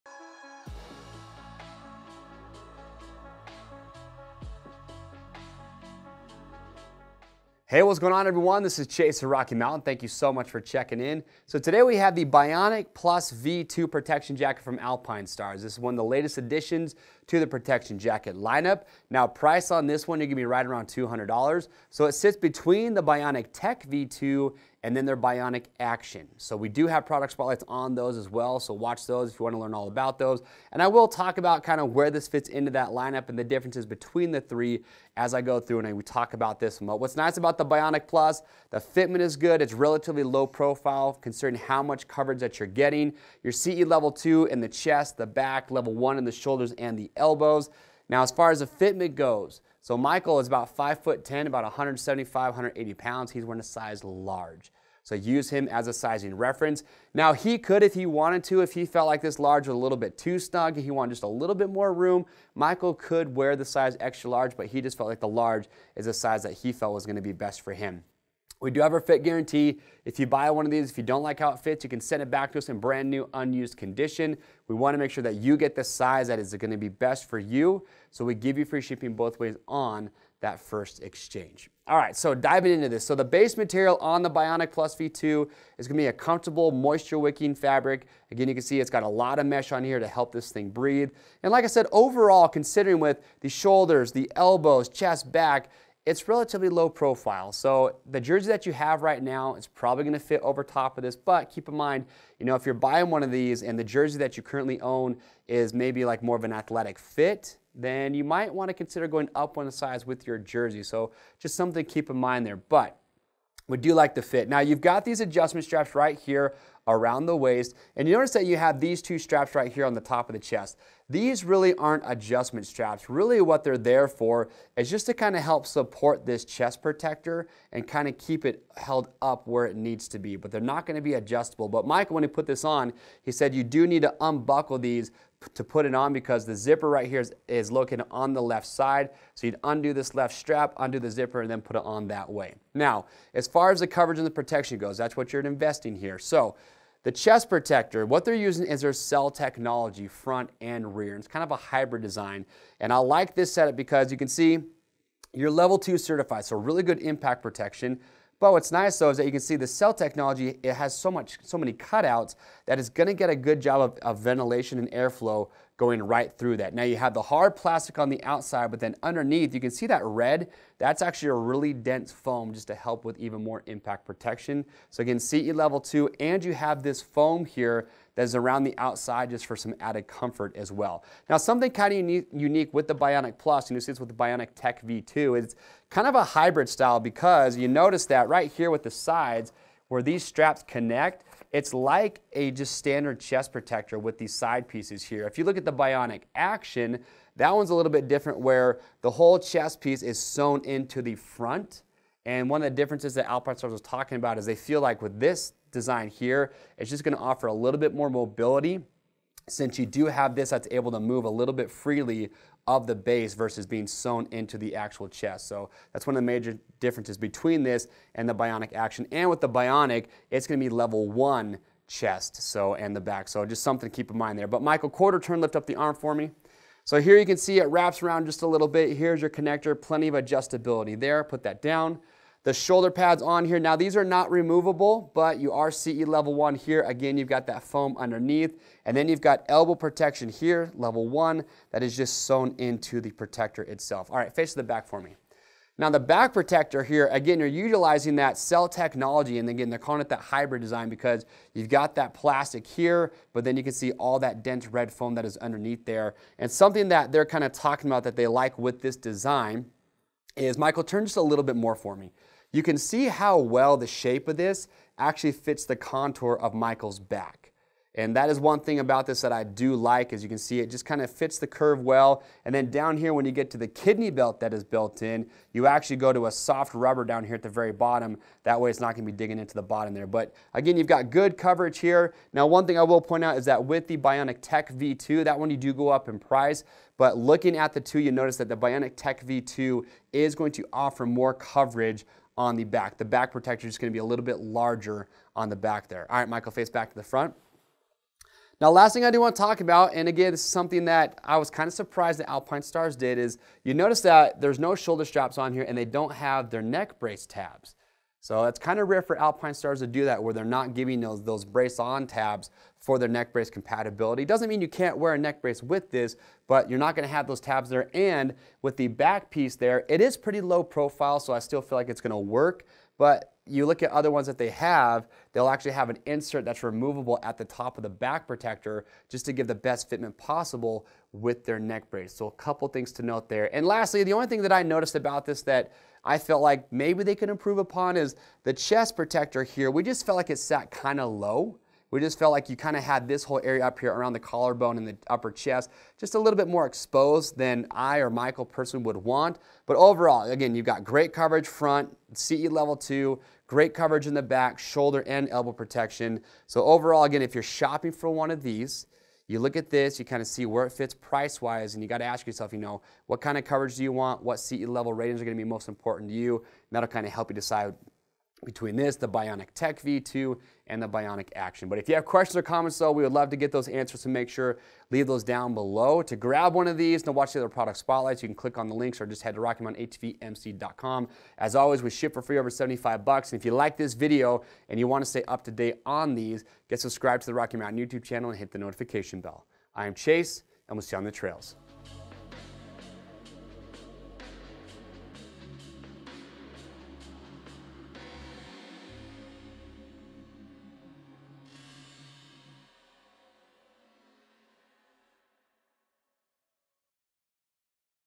Alpinestars Bionic Plus V2 Motocross Protection Jacket
The Alpinestars Bionic Plus V2 Protection Jacket is an ultra-lightweight CE and UKCA certified protection jacket that offers complete upper body protection. Featuring an all-new Level 2 Cell Technology chest protector for ultra-light weight and optimum ventilation. The super-ventilated Level 2 Nucleon Cell & Flex Hybrid Technology back protector is ergonomically engineered for an enhanced adaptive fit. Take a closer look in this product spotlight on the Alpinestars Bionic Plus V2 motocross protection jacket.